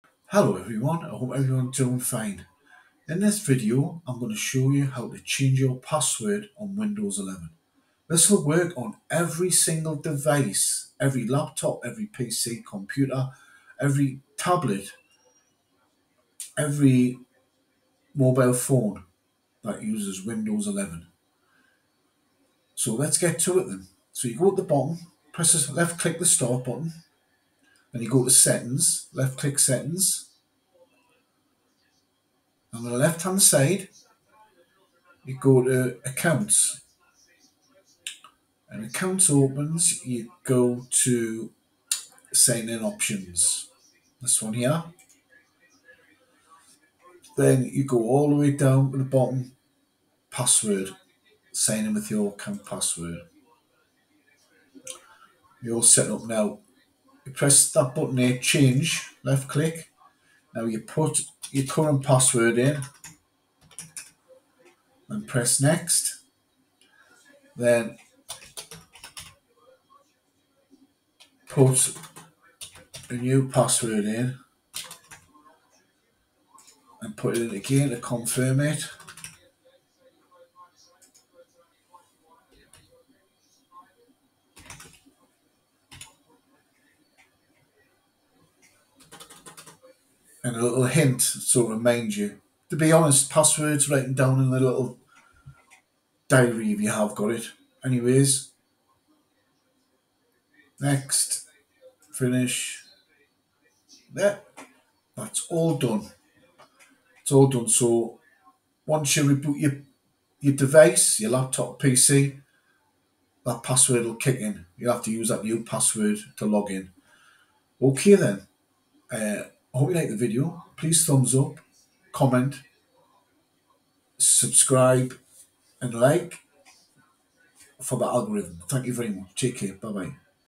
How to Set Password on Windows 11
In This Video, Tutorial, I Guide You, How to Set Password on Windows 11

These instructions can be used for all Laptops, PC, Computers, Desktop and Tablets which use  Windows 11 operating system.

Mr Blinded Guide. △ Tube Buddy Referral